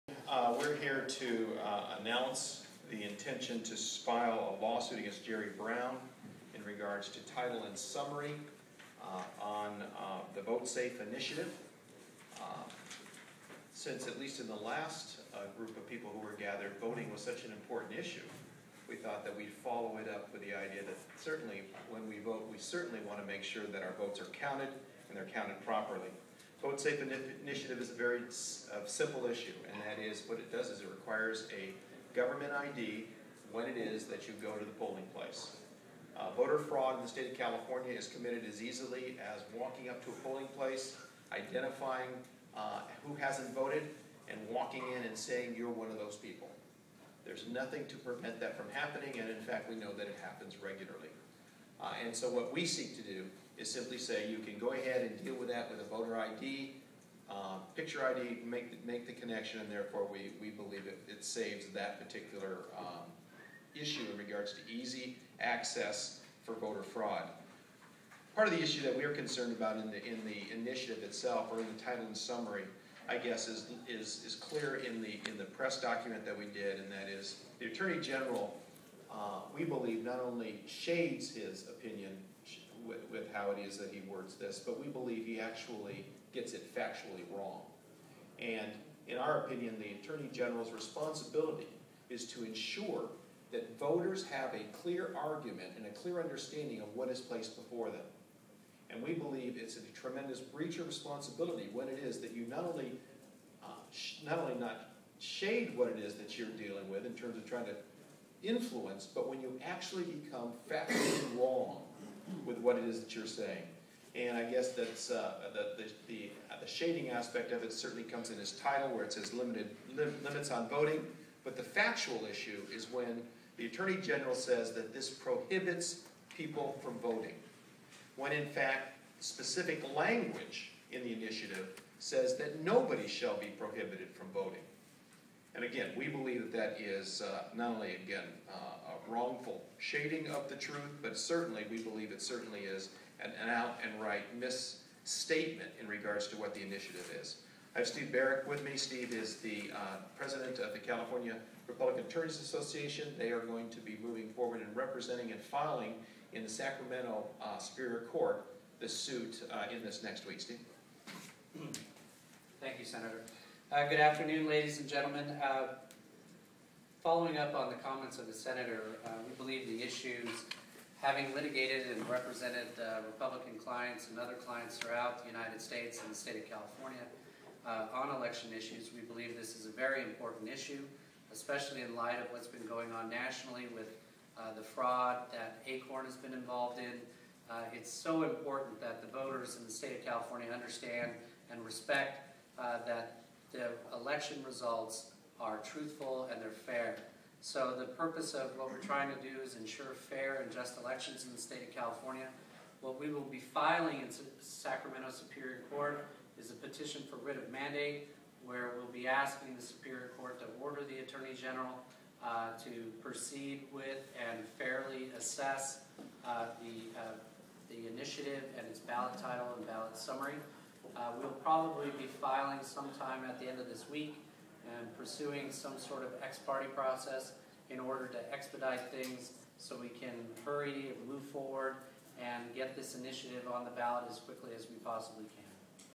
Senator George Runner Sues Jerry Brown
Jerry Brown attempts to mislead voters by drafting an untrue and biased description of the Voter Photo ID initiative prompting a lawsuit by it's author, Senator George Runner